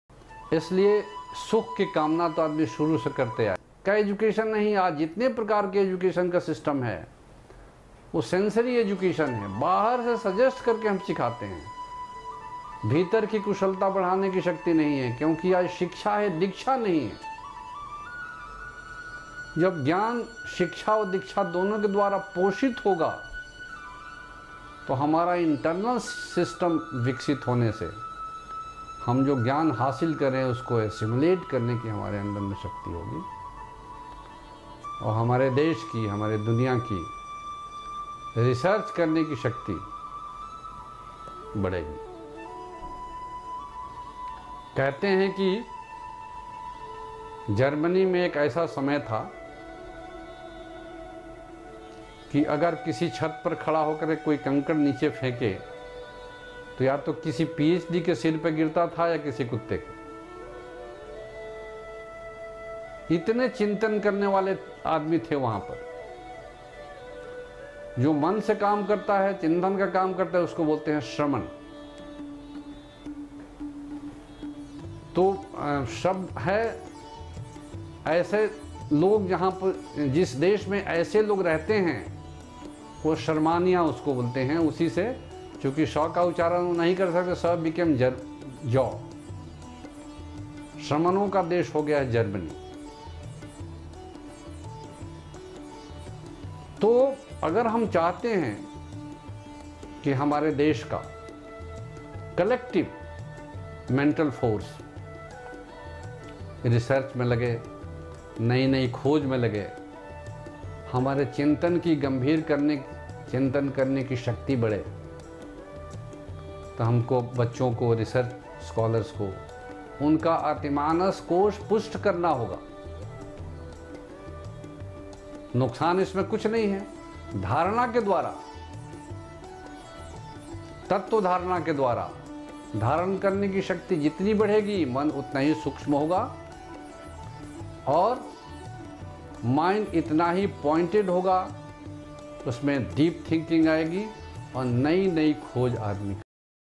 Ananda Marga -Epi 135, सृष्टि चक्र 11 (Cycle of Creation ) by Acharya Rudrananda Avadhuta.
Acaraya Rudrananda Avadhuta talks about the reason for Creation of this universe and its existence and how it all started. How through evolution consciousness advances from microbial life to plant life, from plant to animal and from animal to human life. Lastly how human can go back to its eternal existence through sadhana. After reaching the goal ie. moksha the cycle of creation is complete for each individual.
In this video He talks about Kosa and how to develop different kosa and bring positive changes in our life.